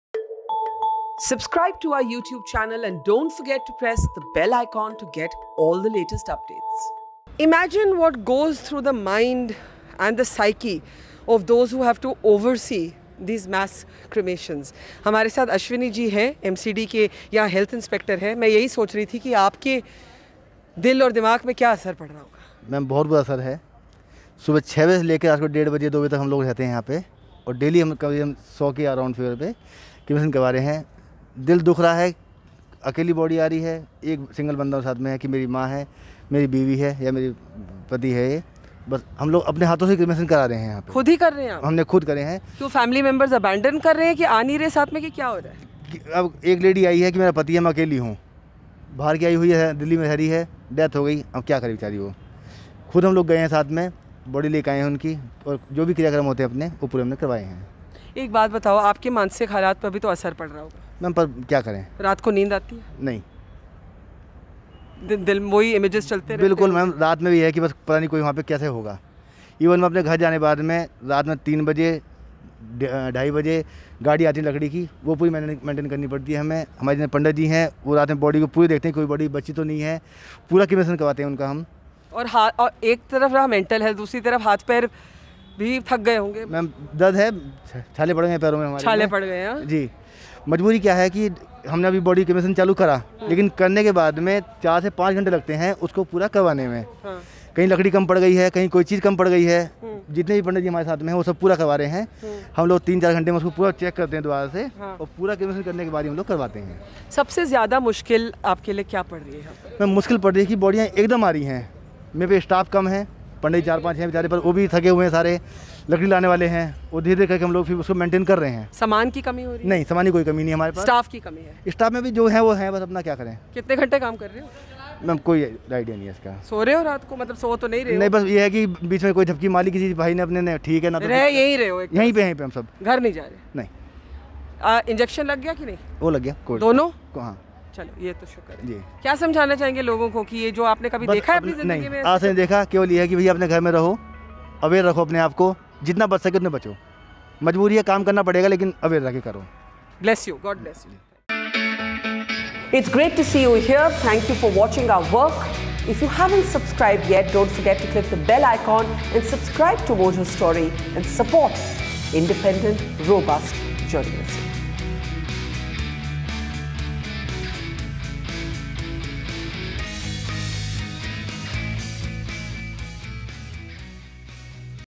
Delhi Becomes City Of Pyres, Crematoriums Run Out Of Staff & Pandits | Barkha Dutt
Barkha Dutt reports from a crematorium in the national capital where bodies of COVID patients are being brought in and cremated 24/7. As funeral sites in Delhi overrun, Mojo Story talked to Mr Ashwini, Health Inspector of MCD, who oversees the mass cremations being done on a daily basis. He told us that these mass cremations certainly affects his mental health and well being as he doesn't even get to sleep properly and goes on to say that the visuals of bodies being cremated keep doing rounds in his mind throughout day and night. He also said that a lot of people who lose their loved ones to COVID come at the funeral site alone and staff at the crematorium help them with getting the body to the pyres and even performing last rites with all traditions and rituals. He added that due to the relentless and tiring work he gets exhausted and even footsores. Amid rush, the cremation site is even facing shortage of staffs and pandits, he tell us. "I no longer go home. I live at the cremation site", he added.